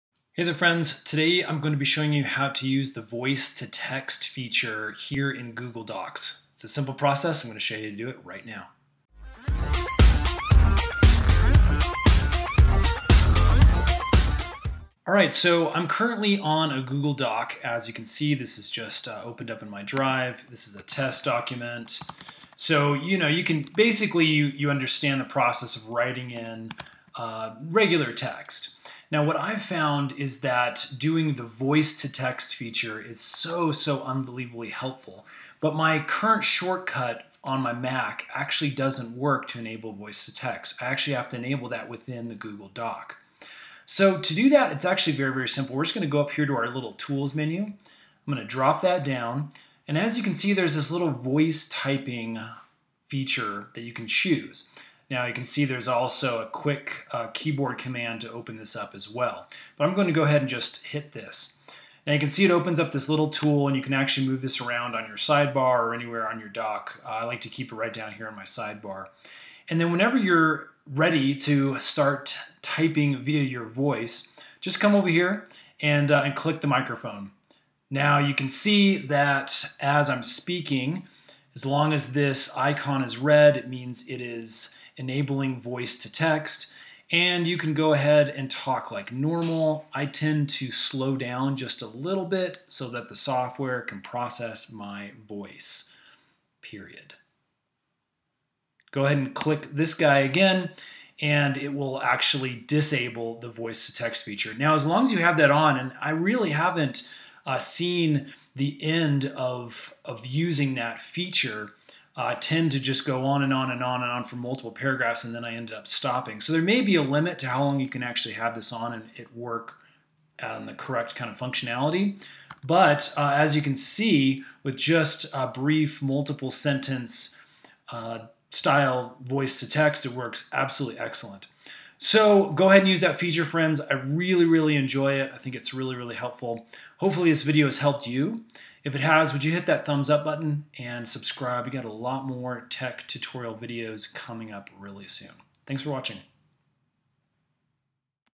Use Voice Recognition in Google Docs: Voice Typing
In this video we show you how to quickly use the voice to text feature in Google Docs. This is a great time saving feature that allows you to enable voice typing using Google's powerful voice recognition software. Once you get the hang of speech to text, you will find you use this feature all the time.